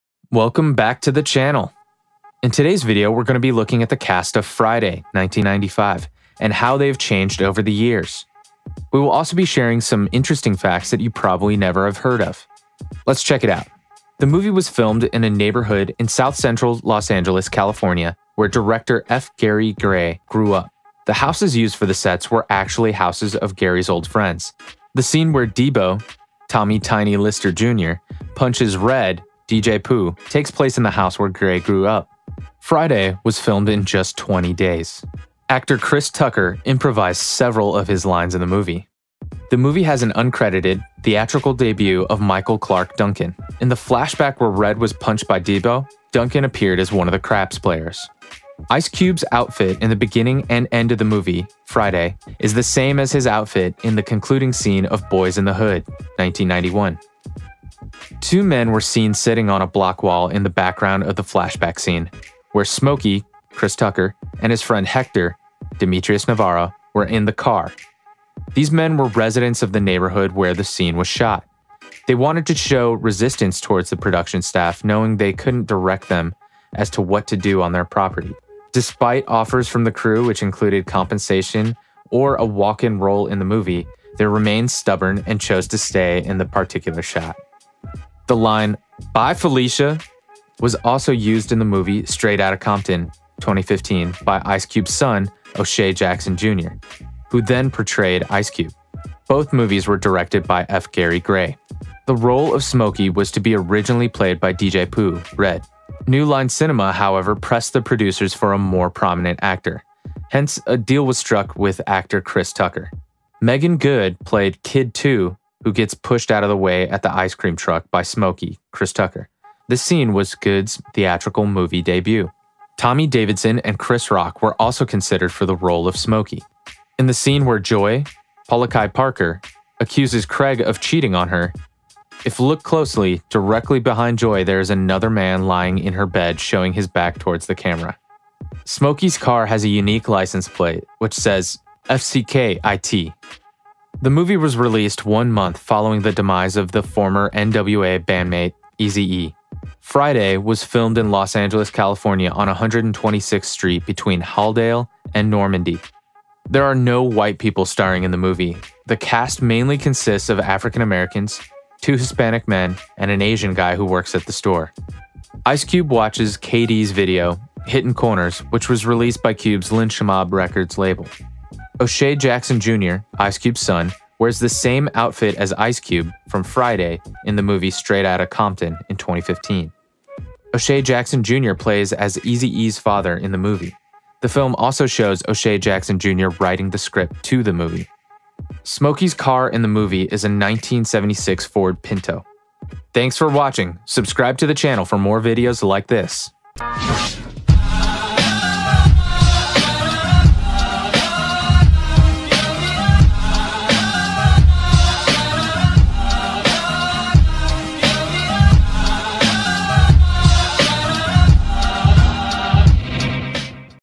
Friday (1995) ★ Then and Now 2022
Here is how the cast of Friday is looking in 2022.